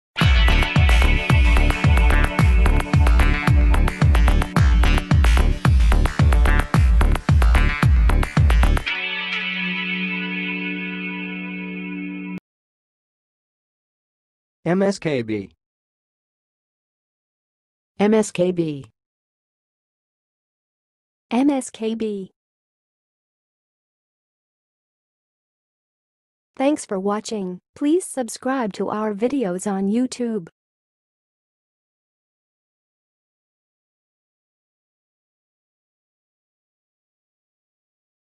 MSKB - HOW TO SAY MSKB?
MSKB - doing Mskb right in 2023

How do you pronounce MSKB?
Can you tell me the correct pronunciation of MSKB?
MSKB Meaning And Definition:
MSKB stands for Microsoft Knowledge Base, a database that contains articles and information related to Microsoft products and technologies.
MSKB Pronunciation:
To pronounce MSKB, say each letter separately: M-S-K-B.
Use MSKB In Your Speech:
1. I found an article about troubleshooting Microsoft Office on the MSKB website
2. The MSKB is a valuable resource for finding solutions to technical issues with Microsoft products
Please answer in comments for the most popular MSKB questions:
1. What is the purpose of the MSKB?
2. How can I access the MSKB?
3. Are the articles in the MSKB regularly updated?